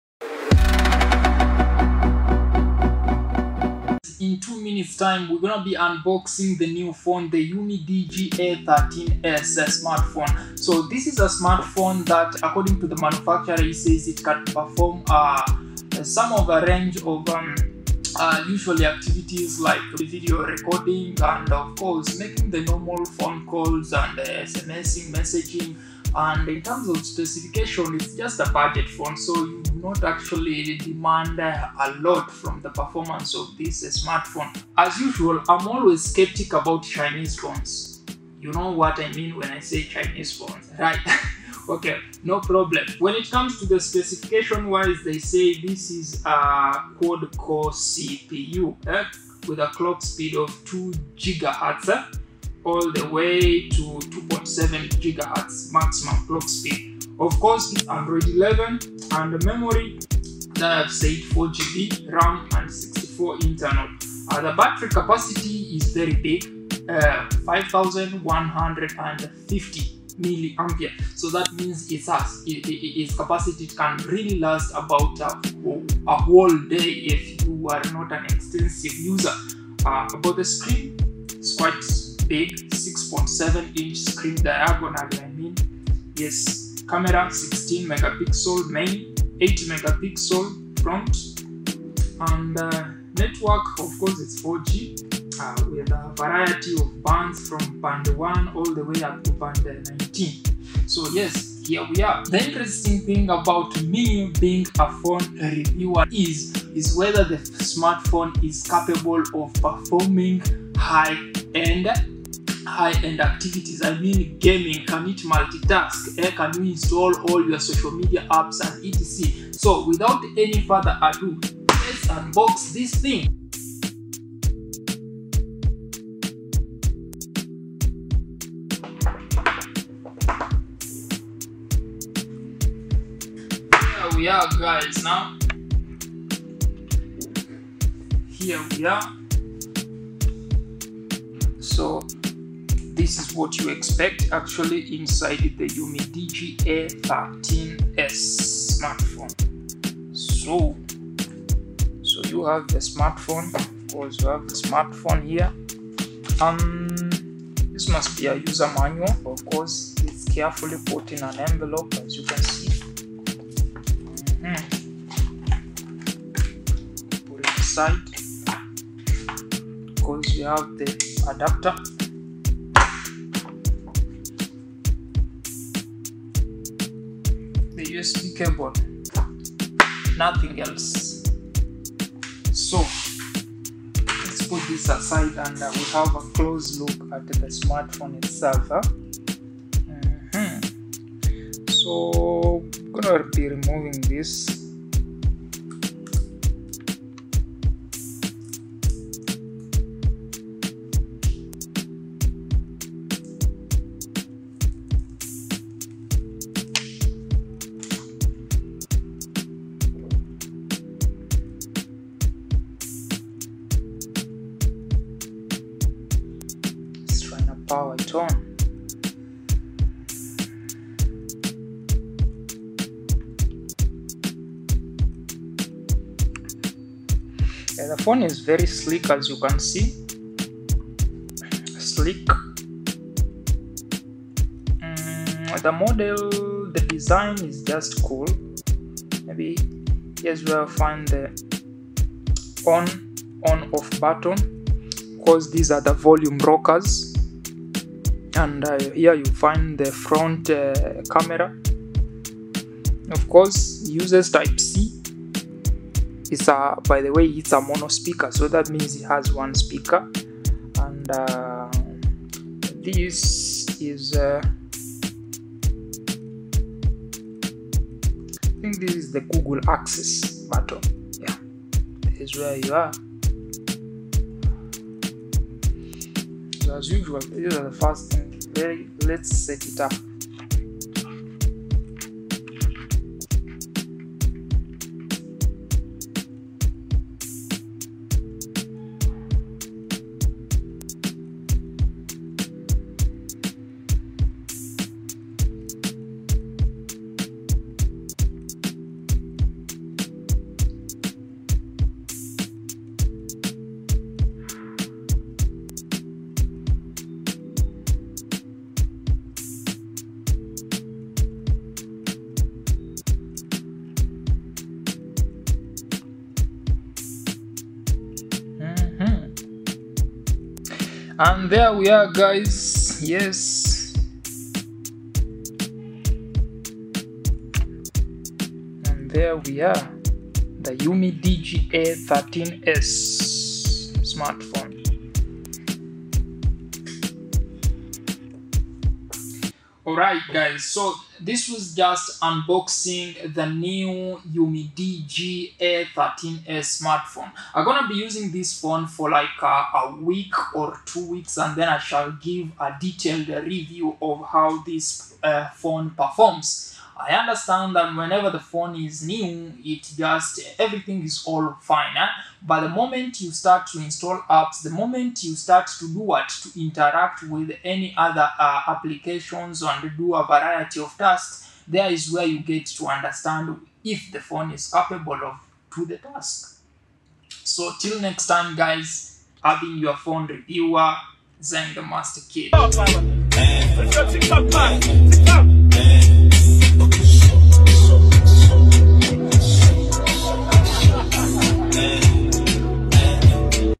UMIDIGI A13S UNBOXING SMARTPHONE
in this video, the UMIDIGI A13S a budget smartphone get unboxed, features like camera, screen properties, storage, battery and speed are discussed as the phone gets powered and  tested from the shop.